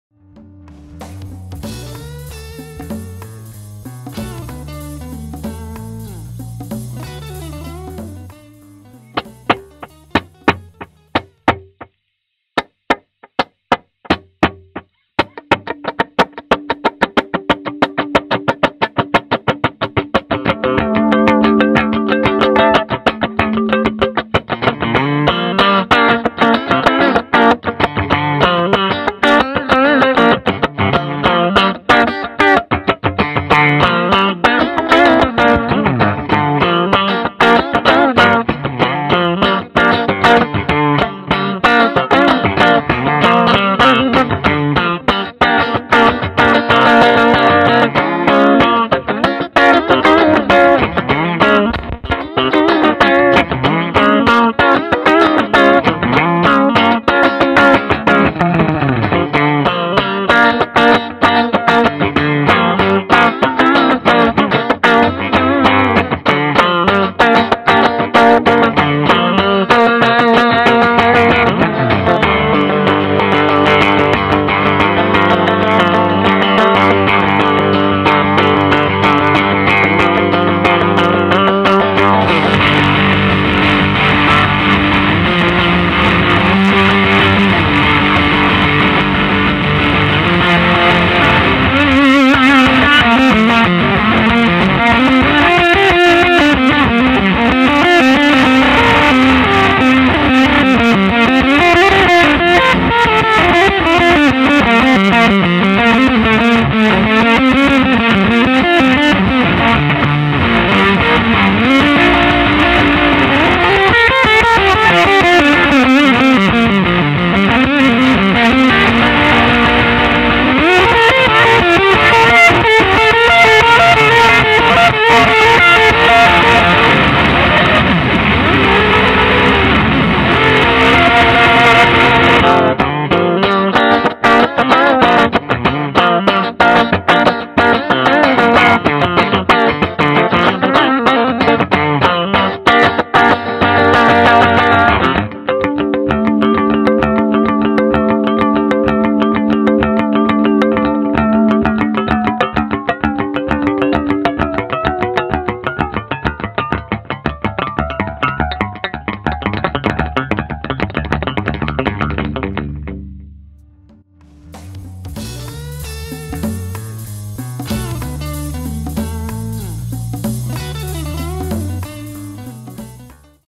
［MASF］ Compressor Pedal experiment Part-1
The EFFECTOR BOOK Vol.27に掲載したMASF実験記事との連動動画です。MASF実験チームがコンプレッサーの活用法を模索、いつものノイズ系ではなく、至極真っ当な音色に仕上がっております。各エフェクターのセッティングなど詳細は誌面で確認してみてください！